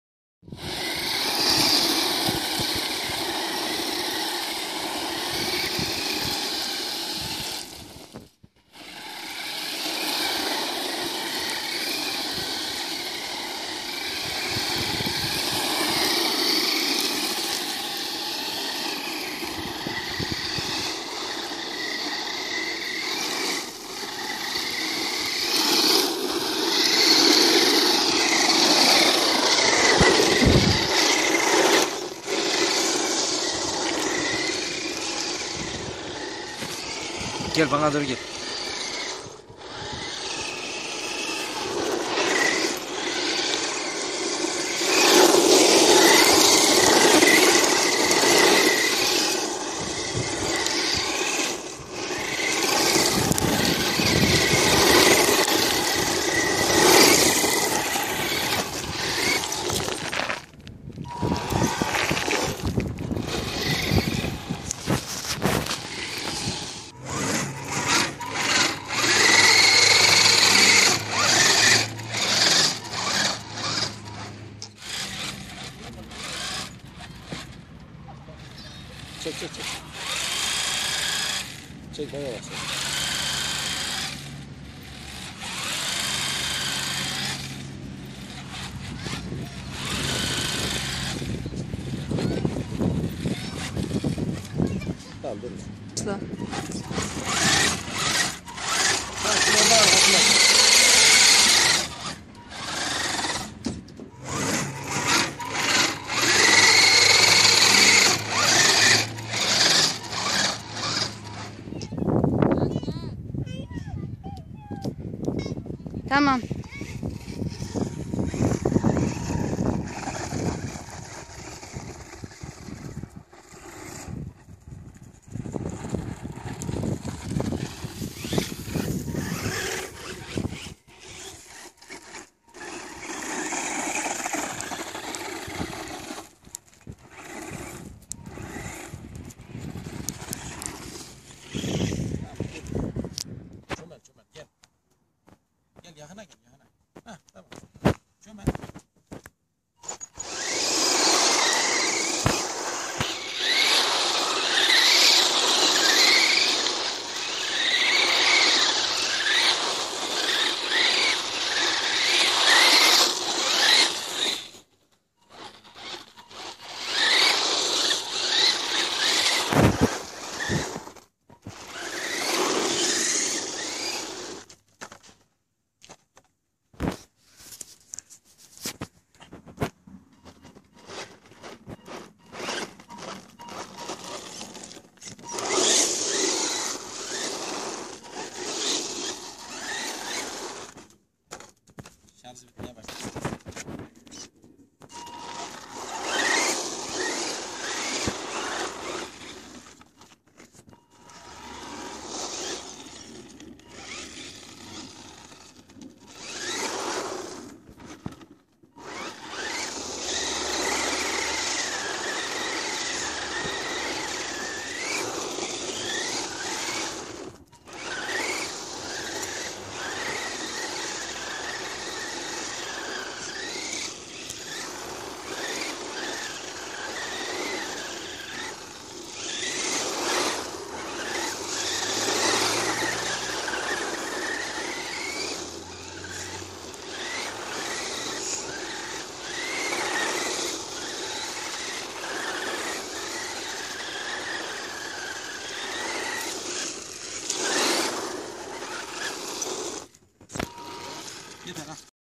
Himoto 1:10 Elektrikli Süper Güçlü Yanlama ve Tırmanma Makinesi
Himoto 1:10 Süper Güçlü Yanlama ve Kaya Tırmanma Makinesi

Himoto Bowie 1:10 Ne kadar güçlü test ettik gerçekten çok güçlü

Traxxxs
Himoto
Driving
Drift
Off Road